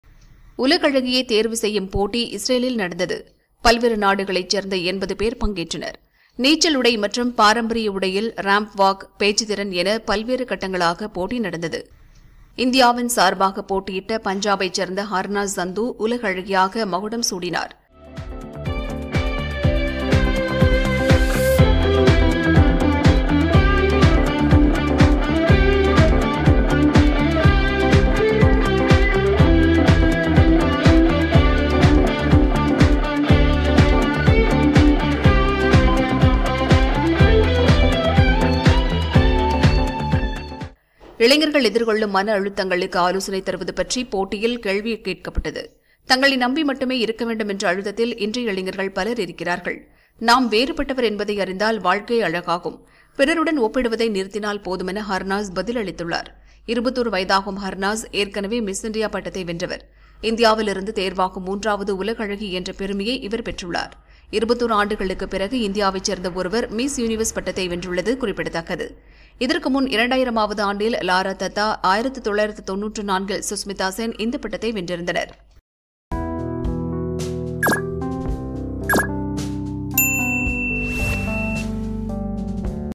21 ஆண்டுகளுக்கு பிறகு வென்றது இந்தியா | Harnaaz Sandhu | Miss Universe 2021
உலக அழகி
பட்டத்தை
வென்றார்